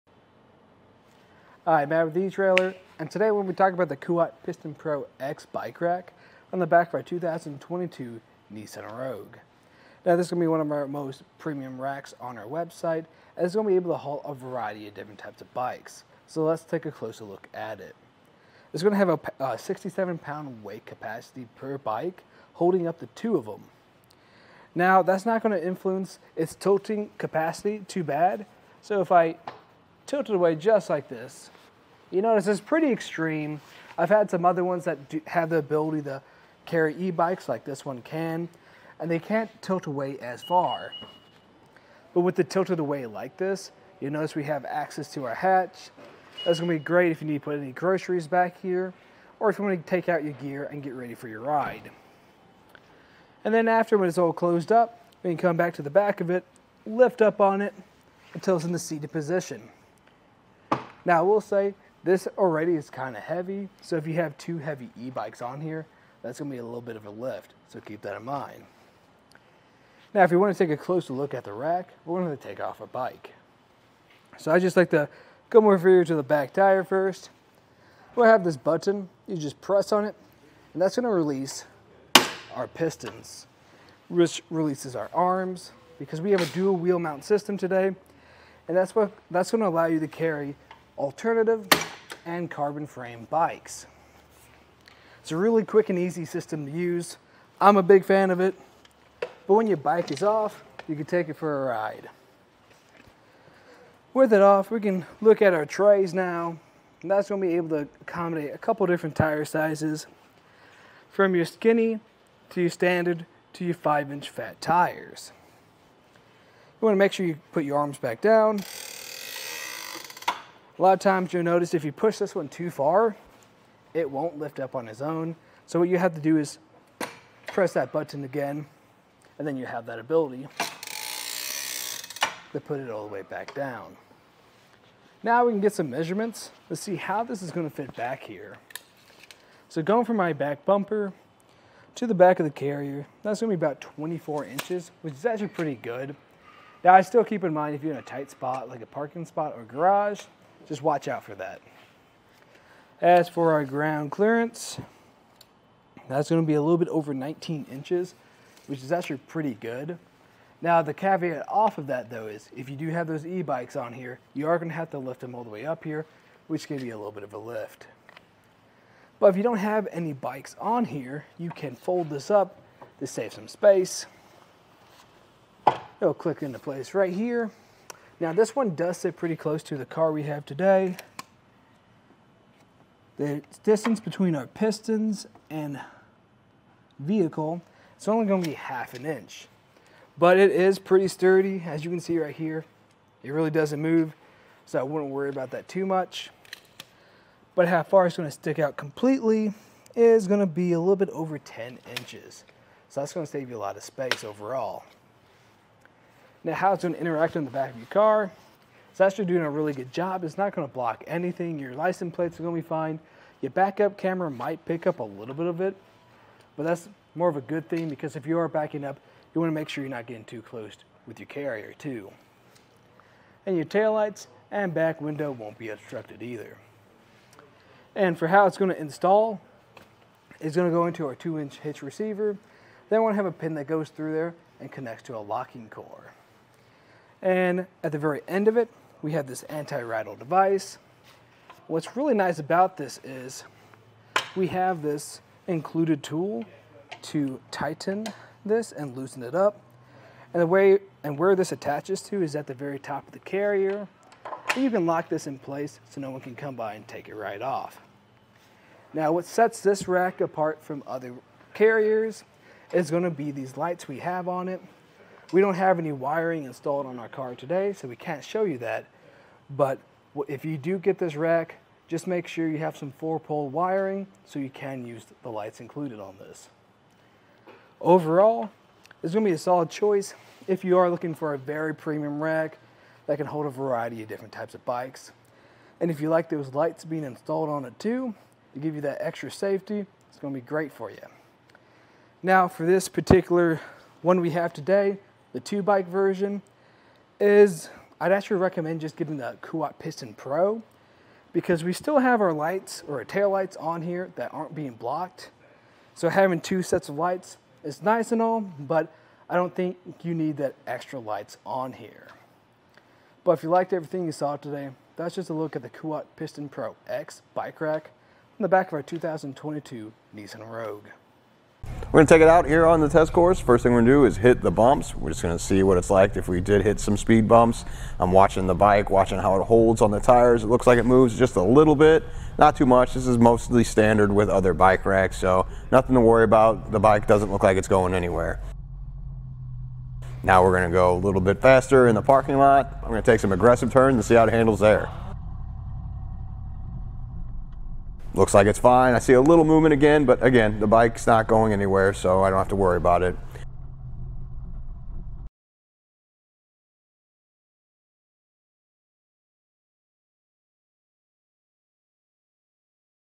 etrailer | Fit Test: Kuat Piston Pro X Bike Rack for 2 Bikes with 2022 Nissan Rogue
Hi, Matt with etrailer,  and today, we're gonna be talking about  the Kuat Piston Pro X bike rack  on the back of our 2022 Nissan Rogue.  Now, this will be one of our most premium racks  on our website, and this is gonna be able to haul  a variety of different types of bikes.  So, let's take a closer look at it.  This is gonna have a 67 pound weight capacity per bike,  holding up to two of 'em.  Now, that's not gonna influence  its tilting capacity too bad.  So, if I tilt it away just like this,  you notice it's pretty extreme.

I've had some other ones that have the ability  to carry e-bikes like this one can,  and they can't tilt away as far.  But with it tilted away like this,  you'll notice we have access to our hatch.  That's gonna be great if you need  to put any groceries back here  or if you wanna take out your gear  and get ready for your ride.  And then after, when it's all closed up,  you can come back to the back of it, lift up on it  until it's in the seated position.  Now, we'll say this already is kind of heavy,  so if you have two heavy e-bikes on here,  that's gonna be a little bit of a lift,  so keep that in mind.

Now, if you wanna take a closer look at the rack,  we're going to take off a bike.  So, I'd just like to come over here to the back tire first.  We'll have this button.  You just press on it, and that's gonna release our pistons,  which releases our arms  because we have a dual wheel mount system today,  and that's gonna allow you to carry  alternative and carbon frame bikes.  It's a really quick and easy system to use.

I'm a big fan of it, but when your bike is off,  you can take it for a ride.  With it off, we can look at our trays now  and that's gonna be able to accommodate  a couple different tire sizes  from your skinny to your standard  to your five inch fat tires.  You wanna make sure you put your arms back down.  (rack creaks)  A lot of times, you'll notice if you push this one too far,  it won't lift up on its own.  So, what you have to do is press that button again,  and then you have that ability  (rack creaks)  to put it all the way back down.

Now, we can get some measurements.  Let's see how this is gonna fit back here.  So, going from my back bumper to the back of the carrier,  that's gonna be about 24 inches,  which is actually pretty good.  Now, I still keep in mind if you're in a tight spot,  like a parking spot or a garage, just watch out for that.  As for our ground clearance,  that's gonna be a little bit over 19 inches,  which is actually pretty good.  Now, the caveat off of that though is  if you do have those e-bikes on here,  you are gonna have to lift 'em all the way up here,  which gives you a little bit of a lift.  But if you don't have any bikes on here,  you can fold this up.  This saves some space.  It'll click into place right here.  Now, this one does sit pretty close  to the car we have today.  The distance between our pistons and vehicle,  it's only gonna be half an inch,  but it is pretty sturdy as you can see right here.  It really doesn't move,  so I wouldn't worry about that too much,  but how far it's gonna stick out completely,  is gonna be a little bit over 10 inches.  So, that's gonna save you a lot of space overall.  Now, how it's gonna interact on the back of your car,  it's actually doing a really good job.  It's not gonna block anything.  Your license plates are gonna be fine.  Your back up camera might pick up a little bit of it,  but that's more of a good thing  because if you are backing up,  you wanna make sure you're not getting too close  with your carrier, too.  And your taillights and back window  won't be obstructed, either.  And for how it's gonna install,  it's gonna go into our two inch hitch receiver.  Then, we'll have a pin that goes through there  and connects to a locking core.  And at the very end of it, we have this anti-rattle device.  What's really nice about this is we have this included tool  to tighten this and loosen it up  and where this attaches to  is at the very top of the carrier.  You can even lock this in place  so no one can come by and take it right off.  Now, what sets this rack apart from other carriers  is gonna be these lights we have on it.  We don't have any wiring installed on our car today,  so we can't show you that.  But if you do get this rack,  just make sure you have some 4-pole wiring  so you can use the lights included on thi